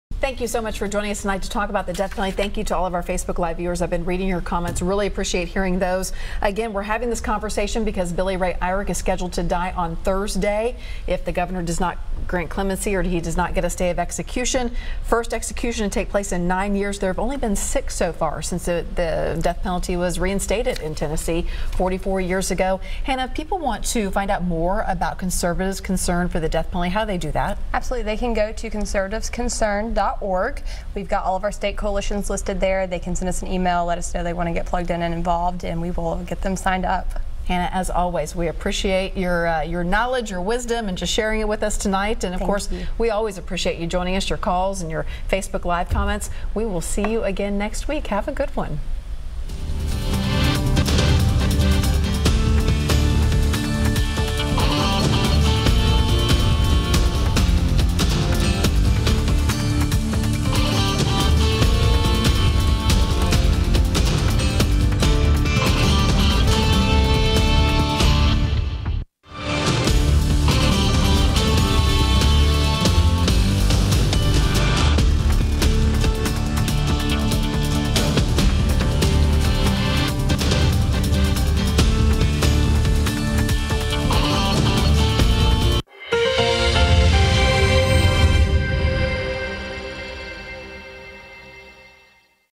The Death Penalty p5
Should the Death Penalty be allowed for those suffering from a mental illness?  59 year old Billy Ray Irick was convicted in the 1985 Rape and Murder of a 7 year old girl. He's scheduled to be executed August 9th.  But opponents state that his Mental illness was not properly considered during his trial.  Hannah Cox with the Conservatives Concerned about the Death Penalty joins Carrie Sharp on discuss why they believe using this method on a person with a diminished state of reality is unjust.